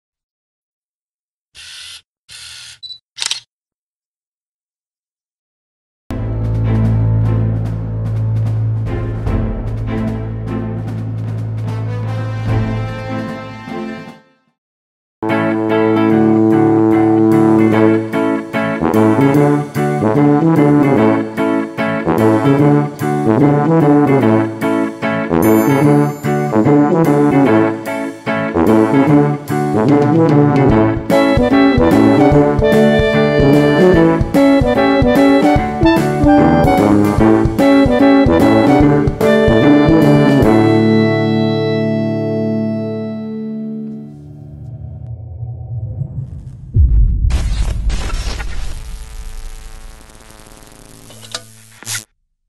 Camaralia te enseña: Mochilas Sachtler
Te traemos la gama de mochilas de la marca Sachtler. En especial, te presentamos el modelo SC300 No dejes escapar la oportunidad para hacer que tu cámara vaya mucho más segura en su transporte.